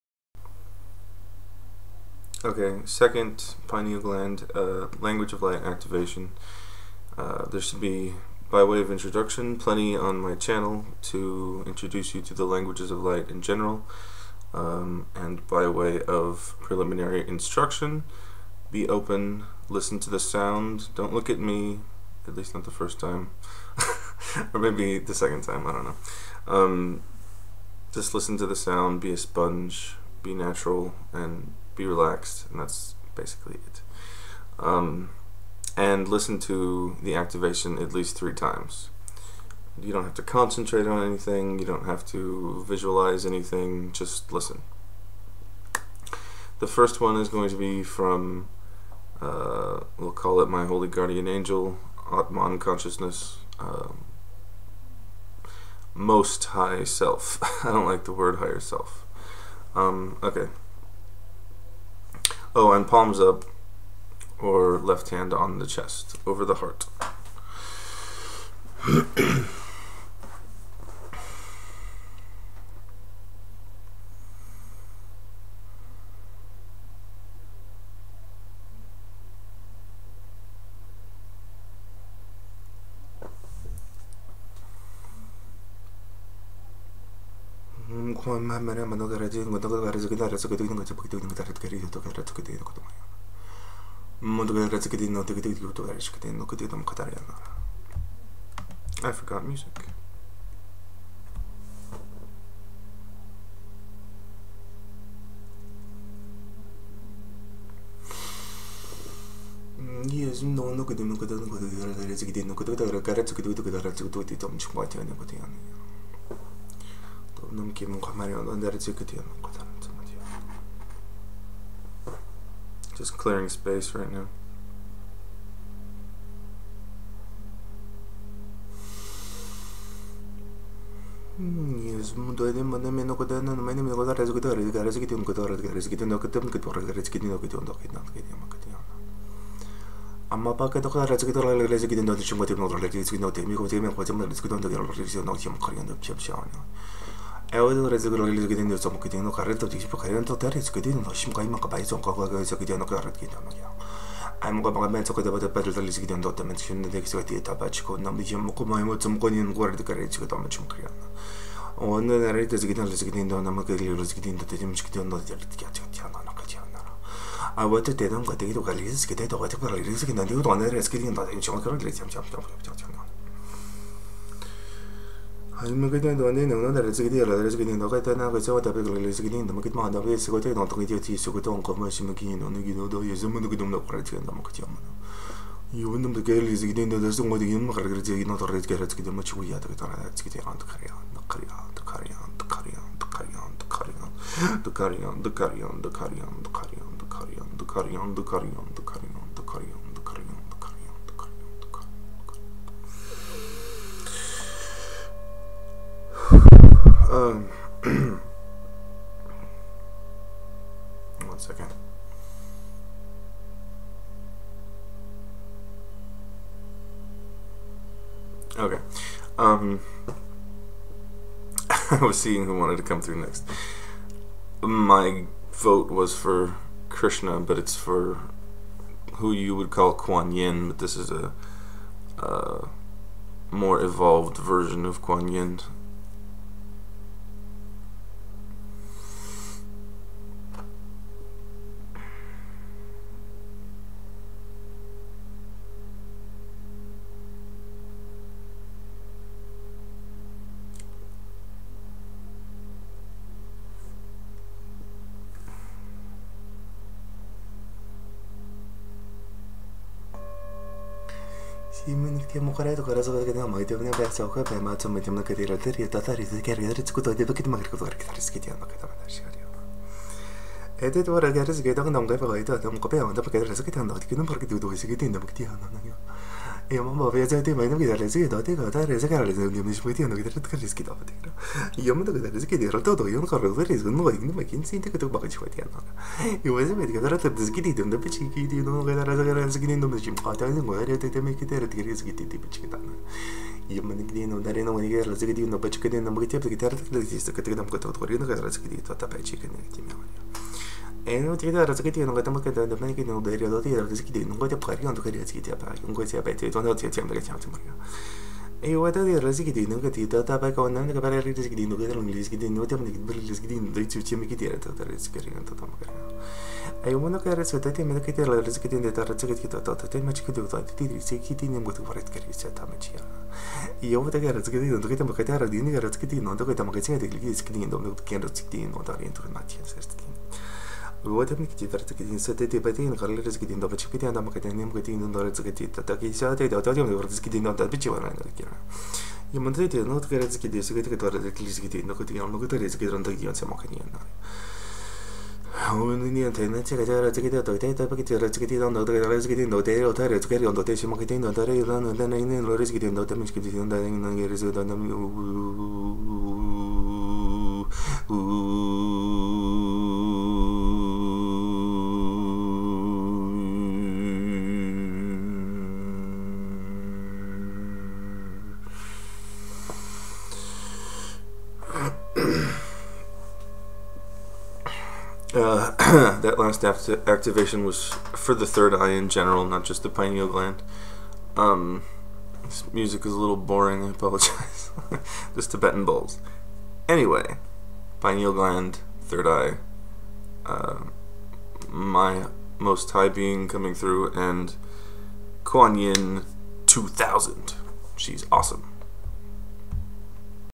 2010-12-28 - Pineal Gland/3rd Eye Activation/Stimulation - My HGA, Ling Hui Sheng Shi(Quan Yin)
Language of light activation for pineal gland from my Holy Guardian Angel(Atman, Logos, Genius, Daemon, etc.)

Language of light activation for 3rd Eye from Ling Hui Sheng SHi(Quan Yin²)